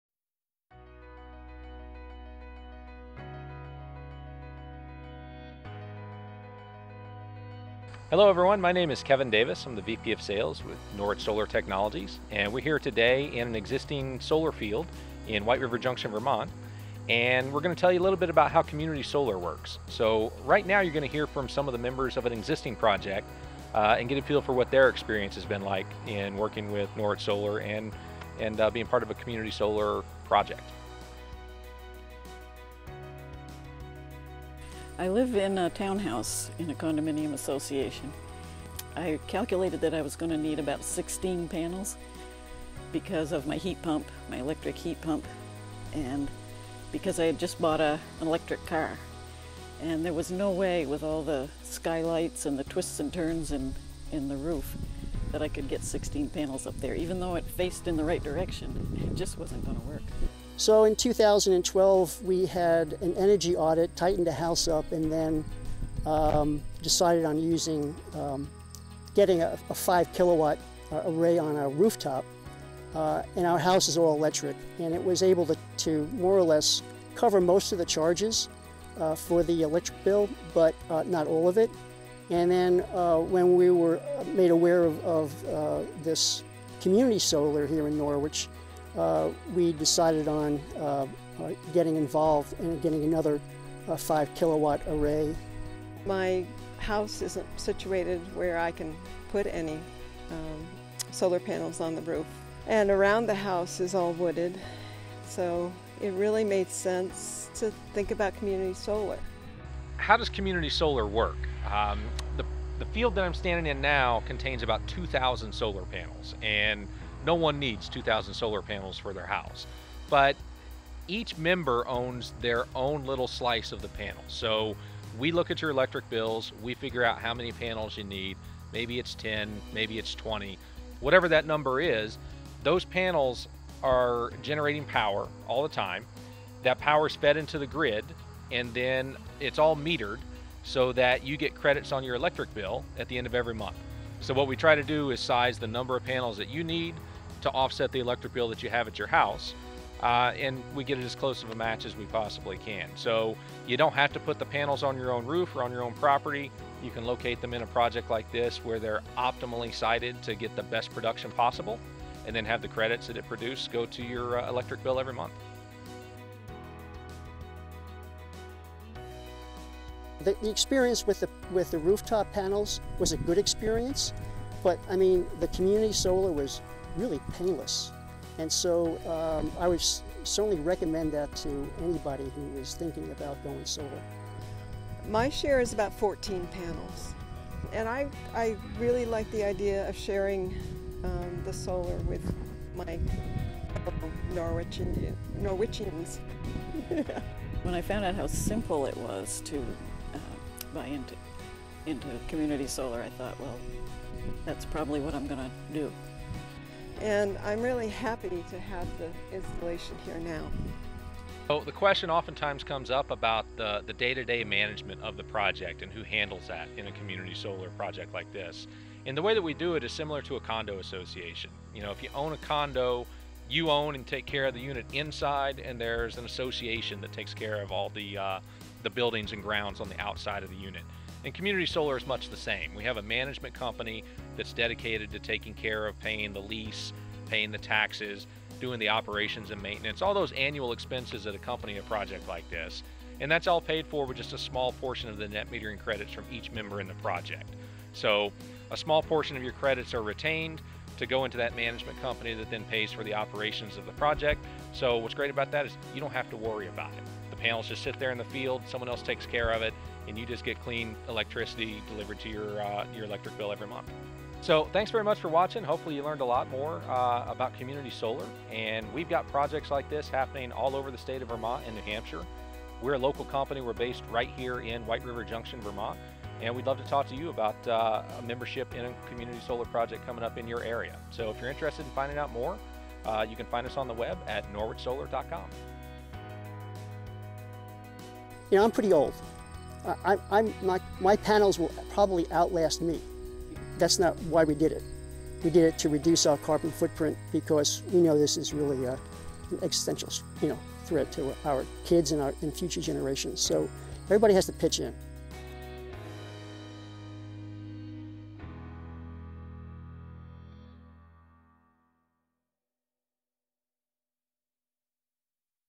Community Solar by Norwich Solar Technologies
A brief overview of Community Solar in Vermont and New Hampshire and testimonials from some members.